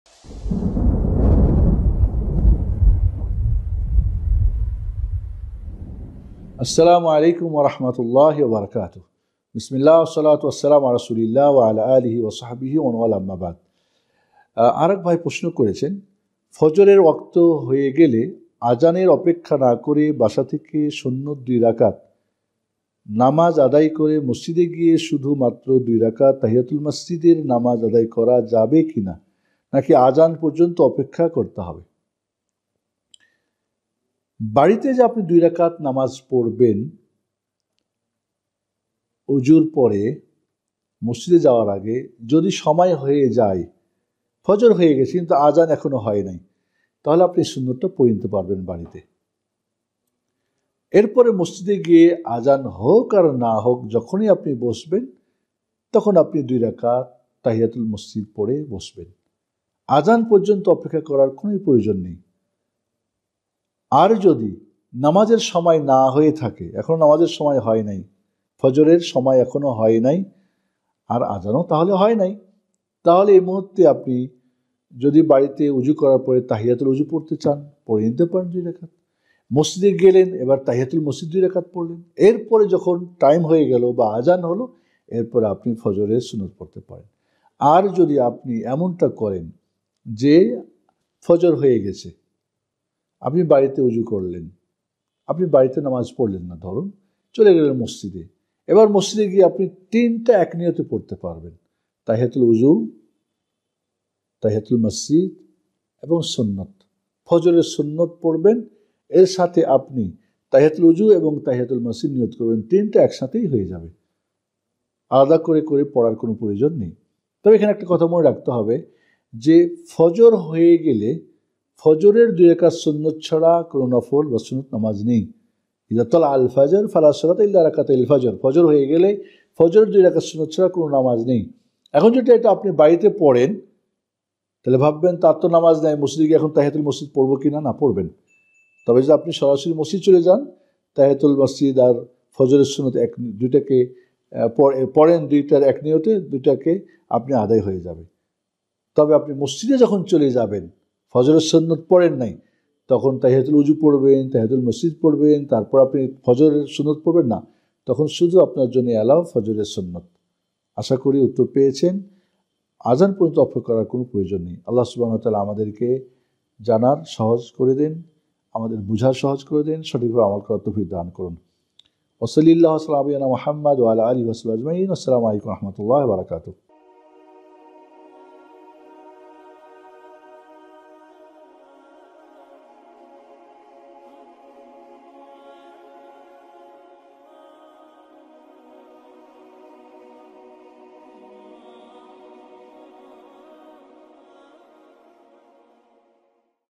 ফজর বাসা থেকে সুন্নত নামাজ পড়ে মাসজিদে গিয়ে শুধু তাইয়াতুল মাসজিদের আদায় করা যাবে│শাইখ সাইফুদ্দিন
👨🏼‍💼Founder, Director & Producer:-  Jobrul Miah
📖 বিষয়ঃ-  ফজর বাসা থেকে সুন্নত নামাজ পড়ে মাসজিদে গিয়ে শুধু তাইয়াতুল মাসজিদের আদায় করা যাবে
🎤 আলোচকঃ-  শাইখ সাইফুদ্দিন
◆◆◆ Video Title ◆◆◆
◆◆◆ Related Hash Tag ◆◆◆
◆◆◆ Related Keyword ◆◆◆
 ◆◆◆ Download videos from this channel ◆◆◆